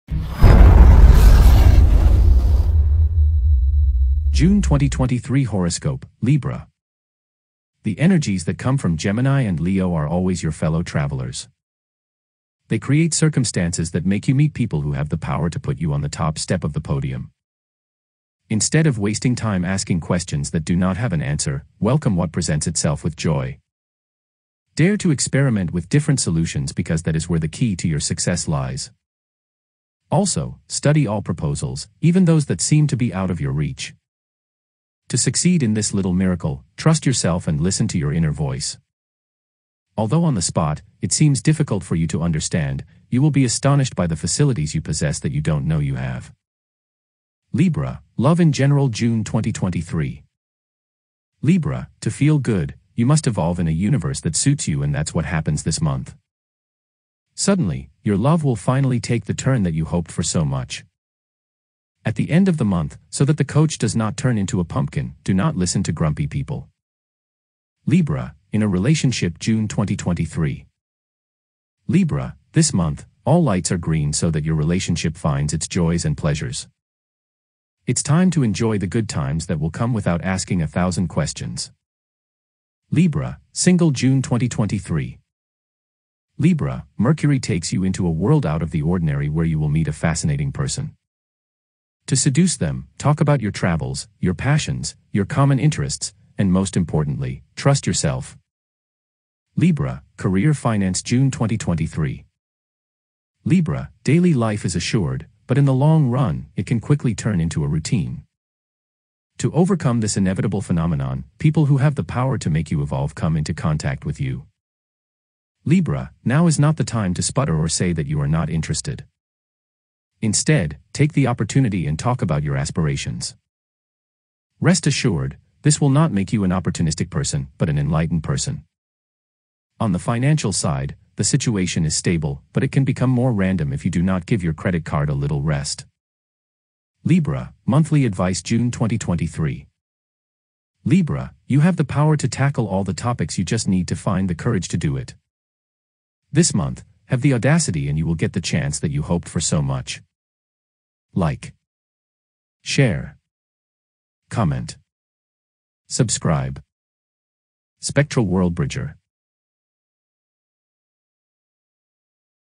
Libra June 2023 Horoscope
June 2023 Horoscope: Libra

The energies that come from Gemini and Leo are always your fellow travelers. They create circumstances that make you meet people who have the power to put you on the top step of the podium. Instead of wasting time asking questions that do not have an answer, welcome what presents itself with joy. Dare to experiment with different solutions because that is where the key to your success lies. Also, study all proposals, even those that seem to be out of your reach. To succeed in this little miracle, trust yourself and listen to your inner voice. Although on the spot, it seems difficult for you to understand, you will be astonished by the facilities you possess that you don't know you have.
Libra: Love in General June 2023
Libra: To feel good, you must evolve in a universe that suits you and that's what happens this month. Suddenly, your love will finally take the turn that you hoped for so much. At the end of the month, so that the coach does not turn into a pumpkin, do not listen to grumpy people.
Libra: In a relationship June 2023
Libra: This month, all lights are green so that your relationship finds its joys and pleasures! It's time to enjoy the good times that will come without asking a thousand questions.
Libra: Single June 2023
Libra: Mercury takes you into a world out of the ordinary where you will meet a fascinating person. To seduce them, talk about your travels, your passions, your common interests, and most importantly, trust yourself.
Libra: Career / Finance June 2023
Libra: Daily life is assured, but in the long run, it can quickly turn into a routine. To overcome this inevitable phenomenon, people who have the power to make you evolve come into contact with you. Libra, now is not the time to sputter or say that you are not interested! Instead, take the opportunity and talk about your aspirations. Rest assured, this will not make you an opportunistic person, but an enlightened person! On the financial side, the situation is stable, but it can become more random if you do not give your credit card a little rest.
Libra: Monthly Advice June 2023
Libra: You have the power to tackle all the topics you just need to find the courage to do it. This month, have the audacity and you will get the chance that you hoped for so much.